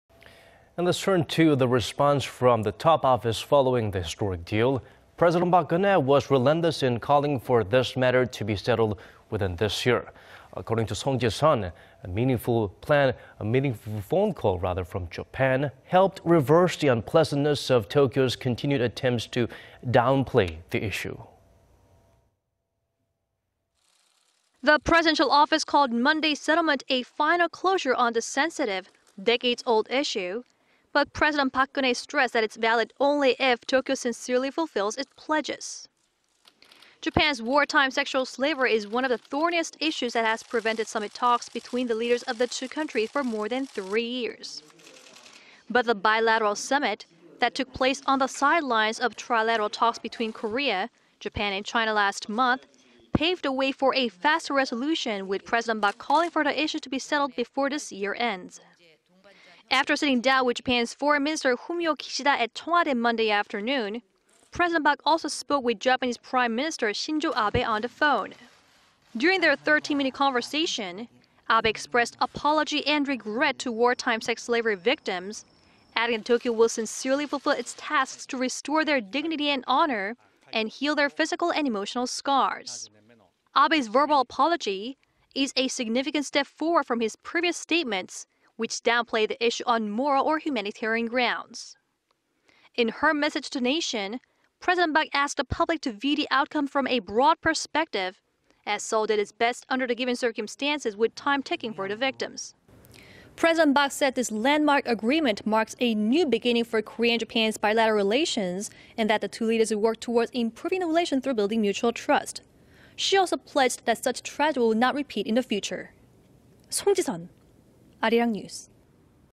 'Sincere fulfillment' key in settling wartime sexual slavery issue: Pres. Park
박 대통령, 일본 측 '성실한 이행' 바라.. 국민에 '대승적' 이해 구해
And now we turn to the response from the top office following the historic deal.
President Park Geun-hye was relentless in calling for this matter to be settled within this year.
According to Song Ji-sun, an meaningful phone call from Japan helped reverse the unpleasantness of Tokyo's continued attempts to downplay the issue.
The presidential office called Monday's settlement final closure on the sensitive, decades-old issue... but President Park Geun-hye stressed that it's valid only if Tokyo sincerely fulfills its pledges.
Japan's wartime sexual slavery is one of the thorniest issues that has prevented summit talks between the leaders of the two countries for more than three years.
But the bilateral summit... that took place on the sidelines of trilateral talks between Korea, Japan and China last month, paved the way for a faster resolution with President Park calling for the issue to be settled before this year ends.
After sitting down with Japan's Foreign Minister Fumio Kishida at Cheong Wa Dae Monday afternoon, President Park also spoke with Japanese Prime Minister Shinzo Abe on the phone.
During their 13-minute conversation, Abe expressed apology and regret to wartime sex slavery victims, adding that Tokyo will sincerely fulfill its tasks to restore their dignity and honor and heal their physical and emotional scars.
Abe's verbal apology... is a significant step forward from his previous statements... which downplayed the issue on "moral" or "humanitarian" grounds.
In her message to the nation, President Park asked the public to view the outcome from a broad perspective, as Seoul did its best under the given circumstances with time ticking for the victims.

"President Park said this landmark agreement marks a new beginning for Korea and Japan's bilateral relations... and the two leaders will work towards improving the relations through building mutual trust. She also pledged no such tragedy will repeat in the future.